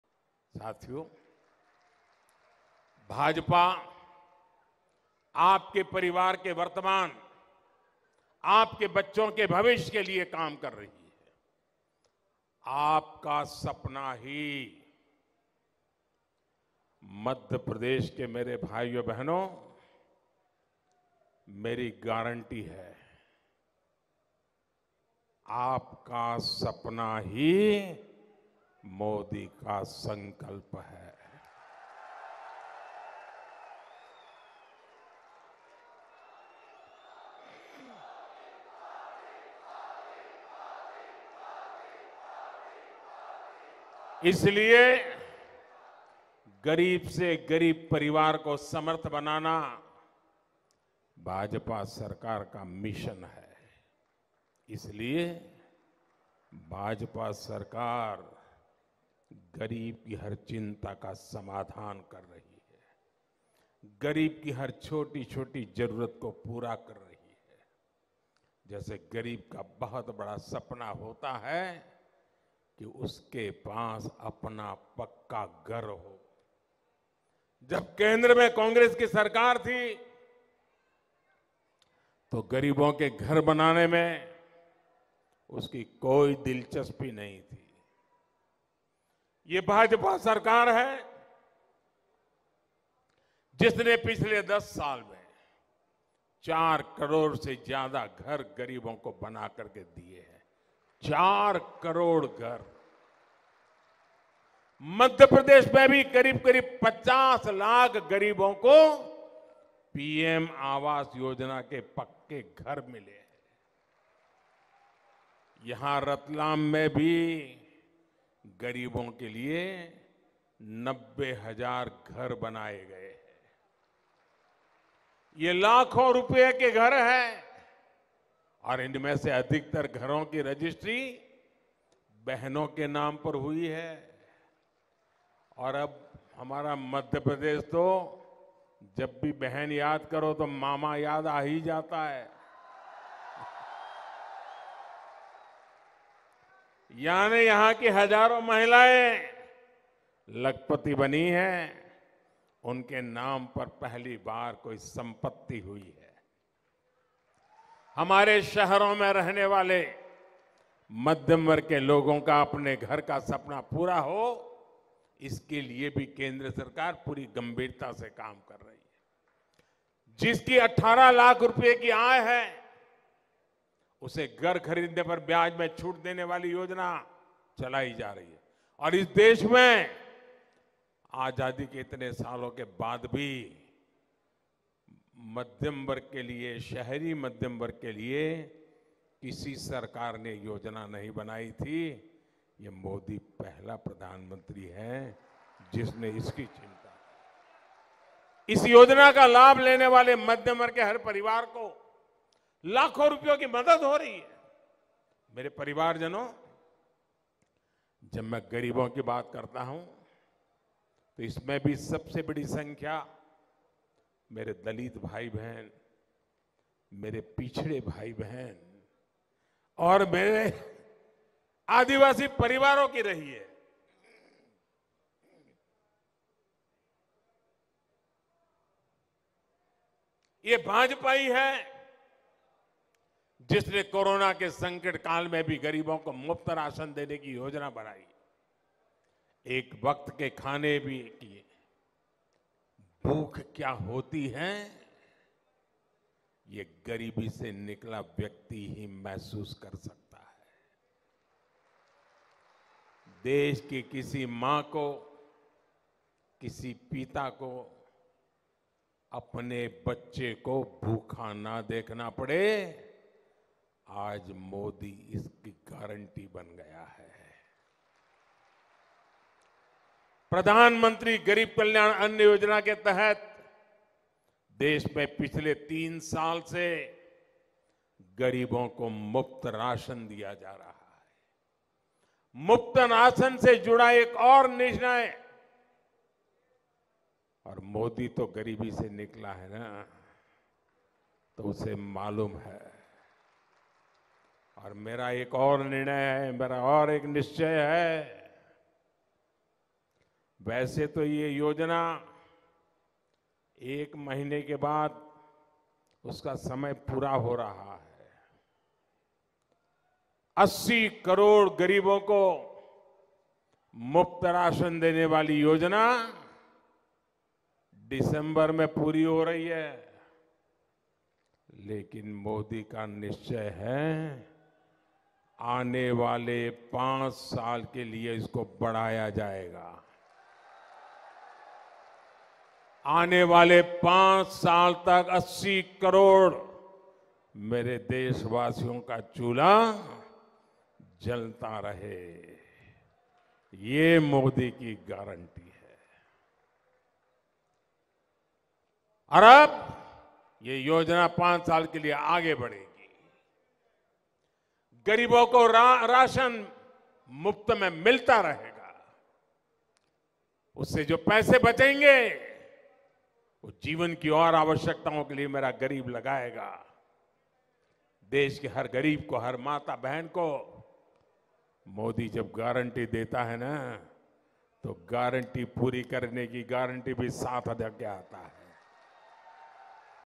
80 करोड़ गरीबों को PM Modi ने दिया Diwali Gift, मुफ्त राशन को लेकर किया बड़ा ऐलान!
Prime Minister Narendra Modi, addressing an election rally in Ratlam, Madhya Pradesh, said that his only resolve is to realise people’s dream and it is BJP’s mission to empower the poorest of the poor. He further said that in order to realise his resolve, 50 lakh poor people were provided houses under PM Awas Yojana and free rations are being provided to 80 crore poor people under the Pradhan Mantri Garib Kalyan Anna Yojana.